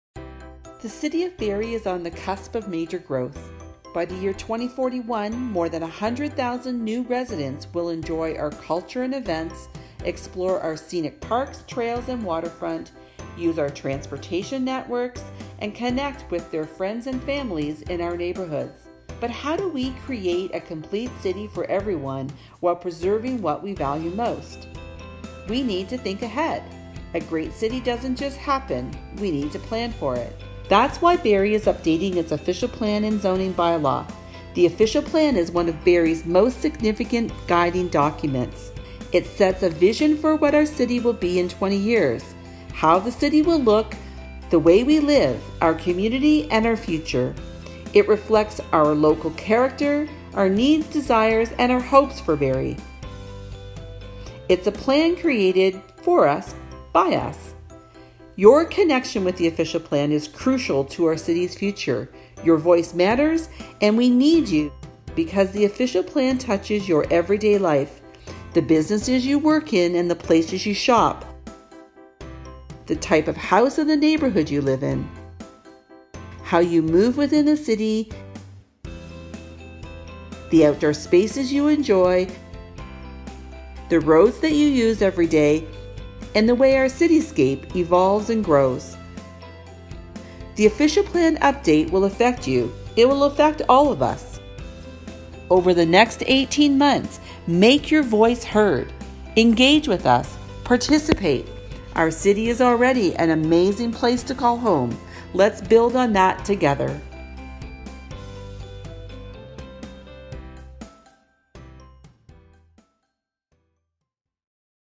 We're creating a new Official Plan!
Andrea Bourrie, Director of Planning & Building Services, introduces the Official Plan Project and how it will affect us all. Over the next 18 months, make your voice heard. Engage with us, and help us build Barrie's future.